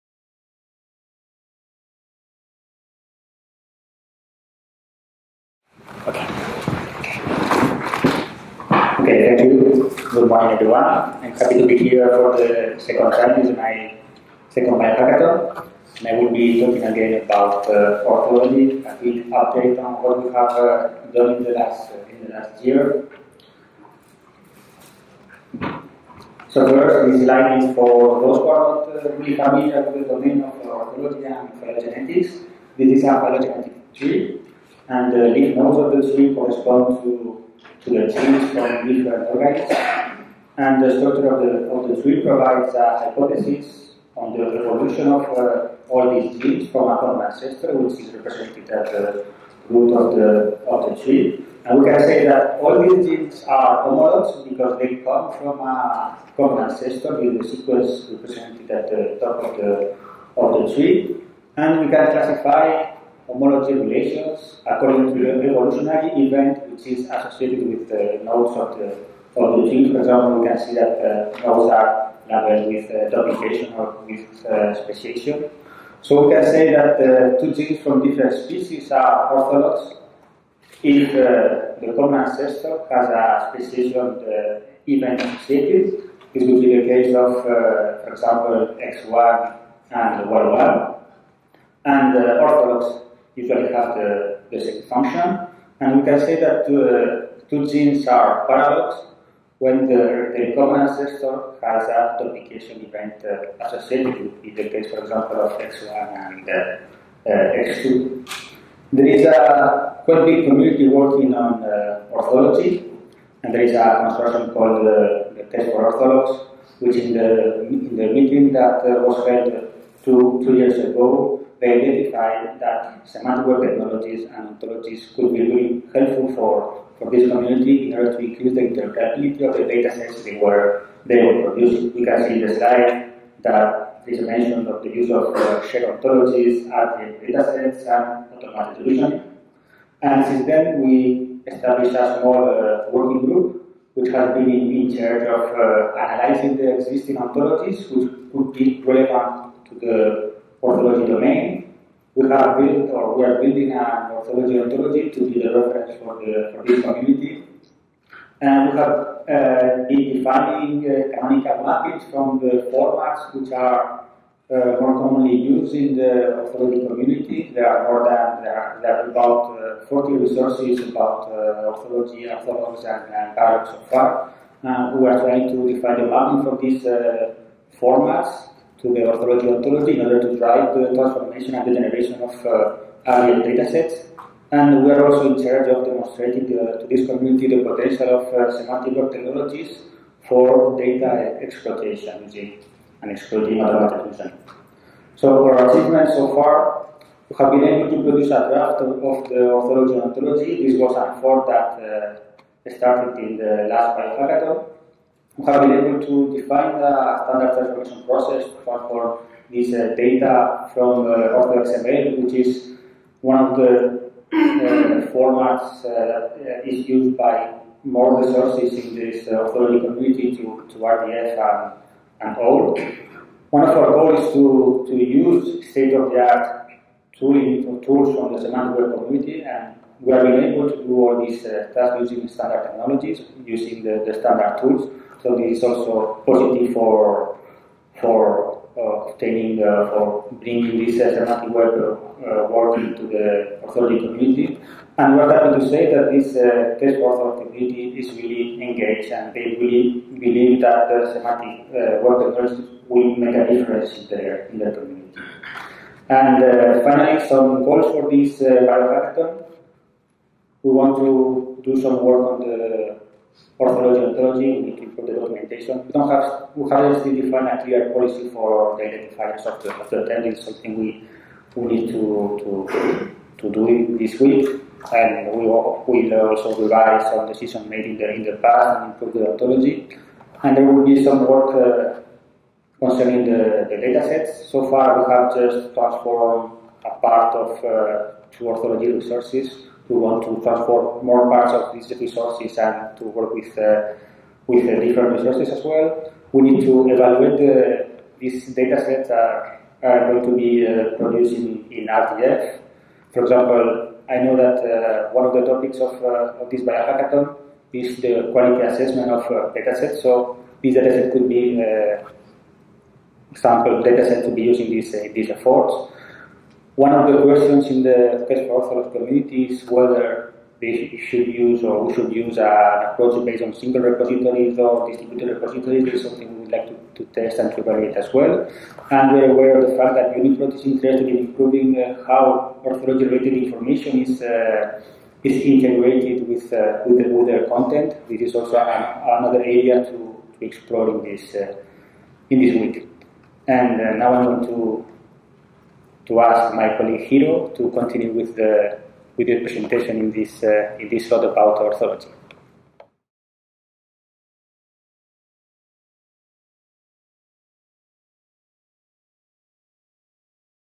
Efforts in the semantic standardization of orthology content@Biohackathon2015
DBCLS BioHackathon 2015 was held at the Nagasaki University and the Luke Plaza Hotel in Nagasaki, Japan.

Public symposium of the BioHackathon 2015 will be held at the Ryojun Kaikan, Atomic Bomb Disease Institute, Nagasaki University, Nagasaki, Japan, on Sep 13 (Sun).

In this talk, Jesualdo Tomás Fernández-Breis (University of Murcia, Spain) makes a presentation entitled "Efforts in the semantic standardization of orthology content". (6:01)